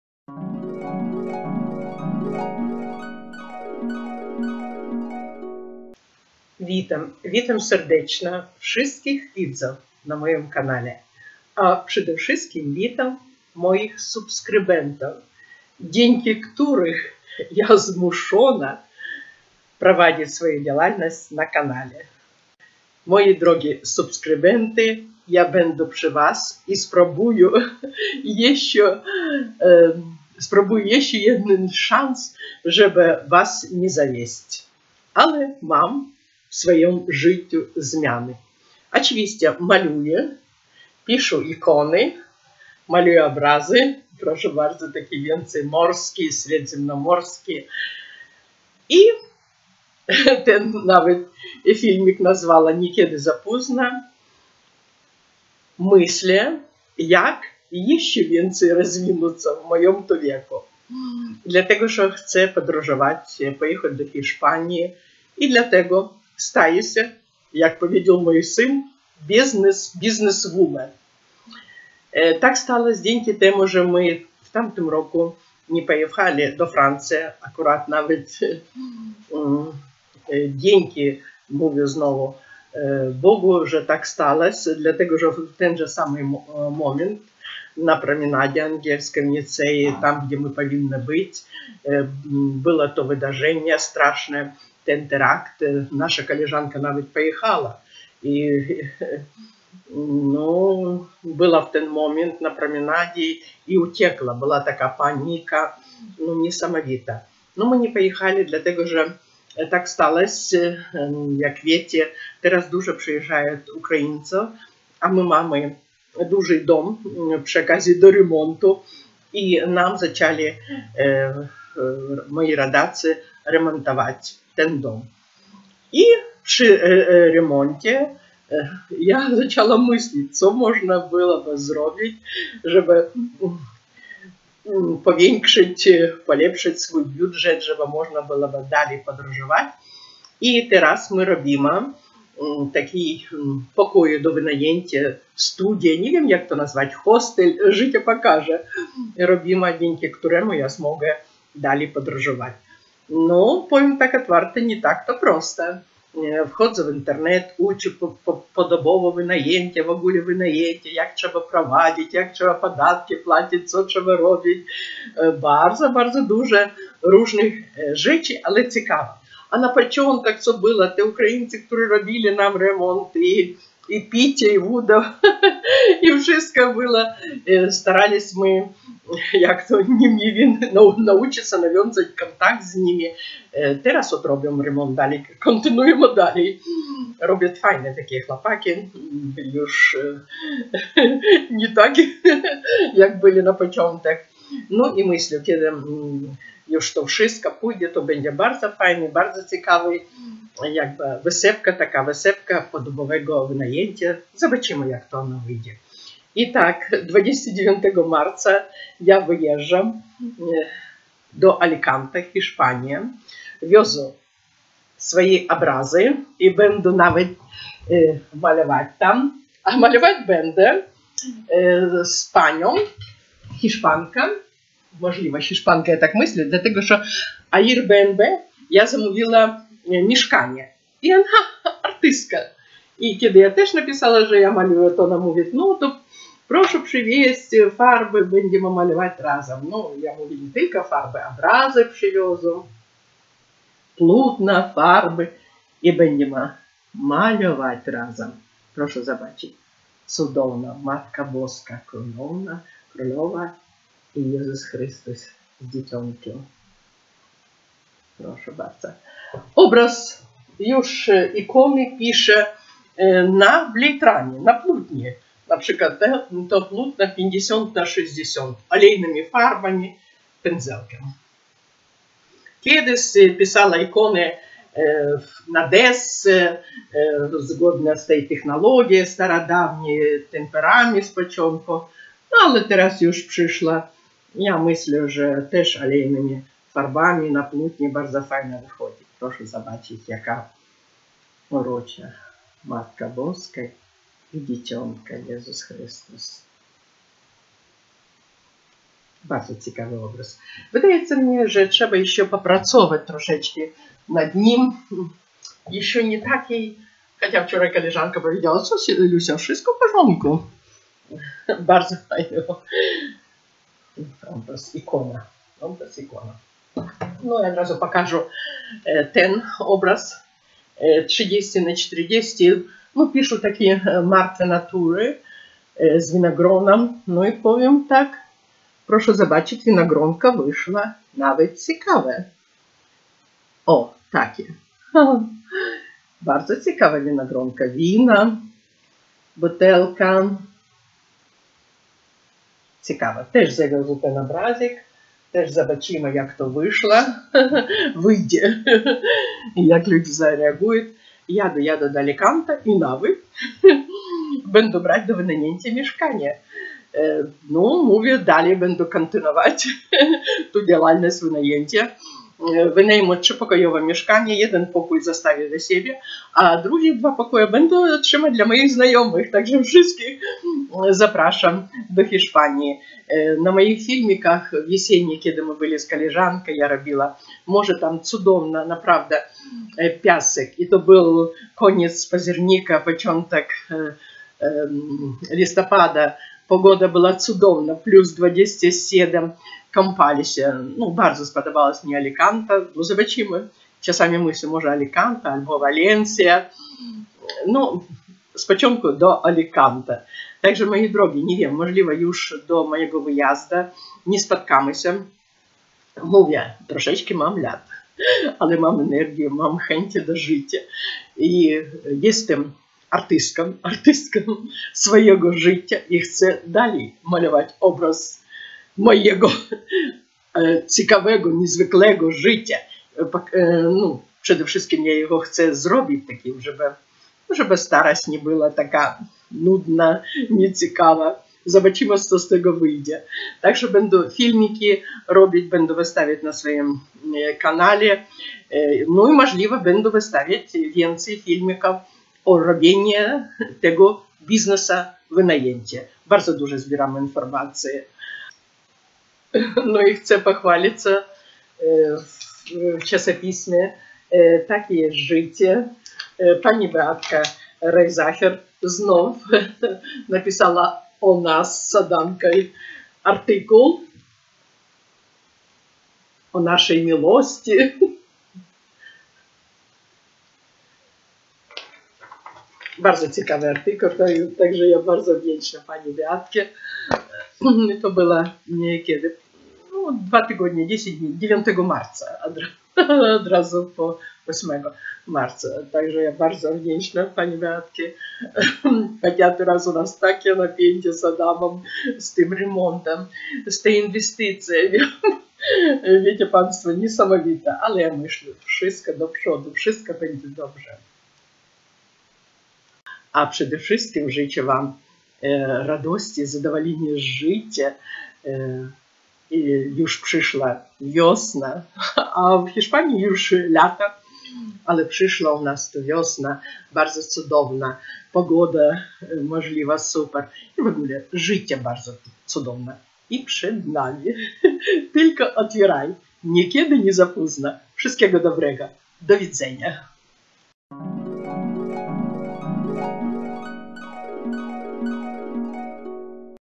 Nigdy nie jest za póżno.
Na wideo " Nigdy nie jest za póżno." malarka prezentuje swoję pracy i mówi , jak stać artystą swego życia.